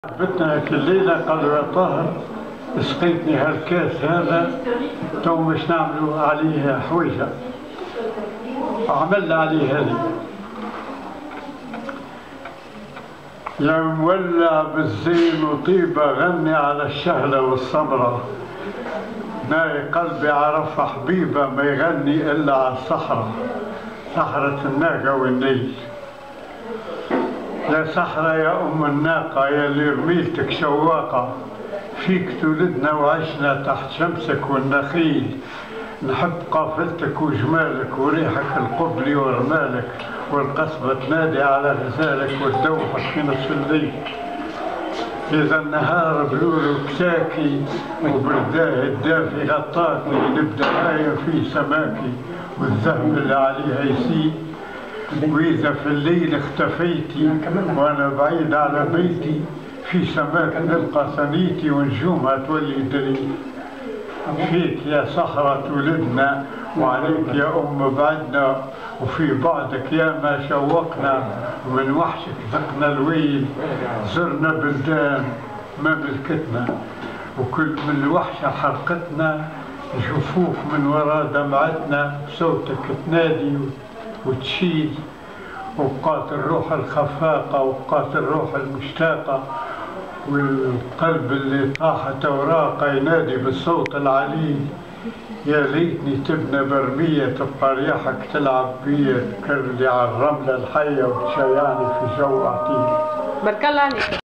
Les frères Jamoussi
Abdelaziz Jamoussi récite un des poèmes de son frère le chanteur, le compositeur et le poète Mohamed Jamoussi